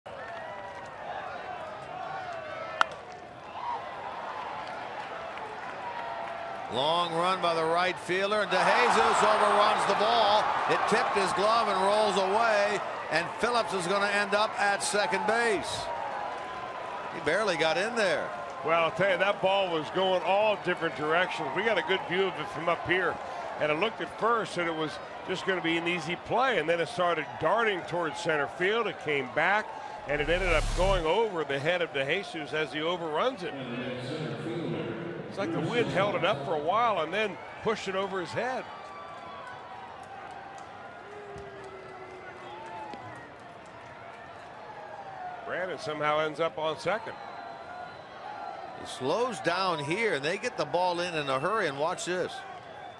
2012/09/19 Phillips reaches on error
CIN@CHC: Phillips reaches on DeJesus' two-base error